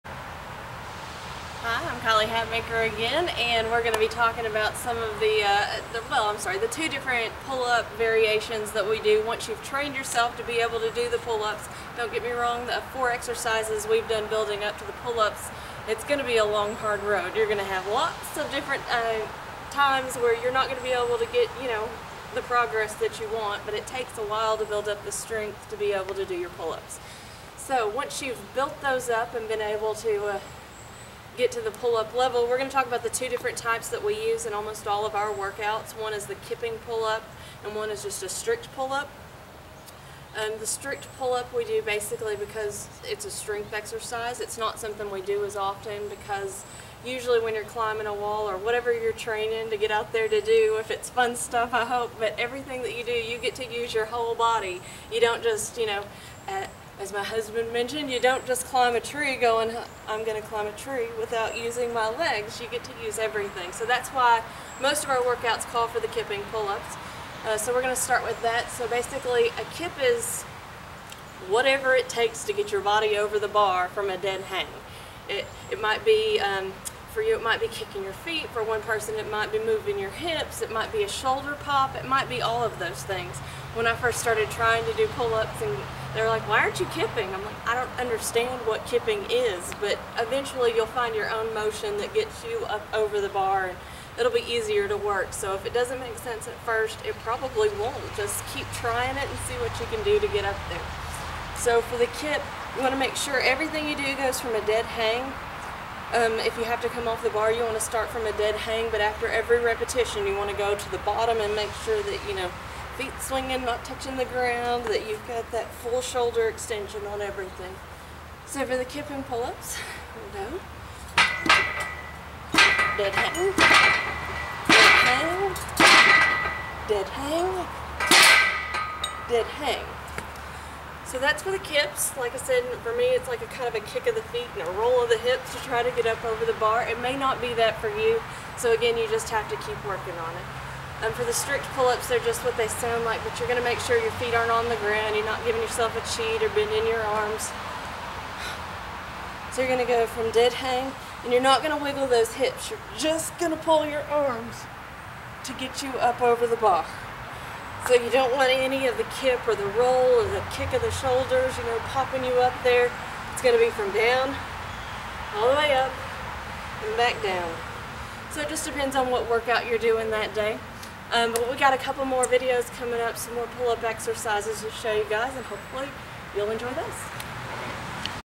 Kipping Pull-ups and Strict Pull-ups
Kylie Hatmaker demonstrates the difference in Kipping Pull-ups and Strict Pull-ups.  More exercises and workouts available in her upcoming book, "She's Tough."